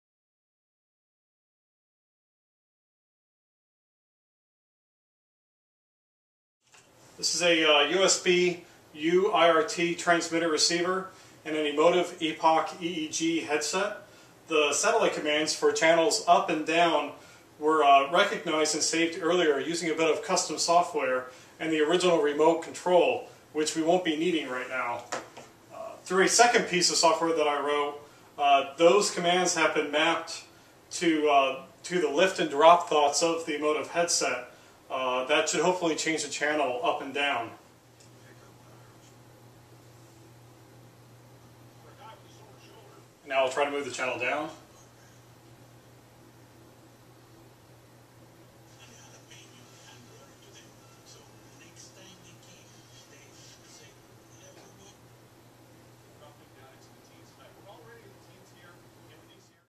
Mind Controlled TV Remote
An attempt at a Mind Controlled TV Remote application using the Emotiv Epoc EEG headset and the USB UIRT infrared transmitter/receiver.  The hand motions were used during thought training in the Emotiv system to assist in recognizing the 'lift' and 'drop' thoughts.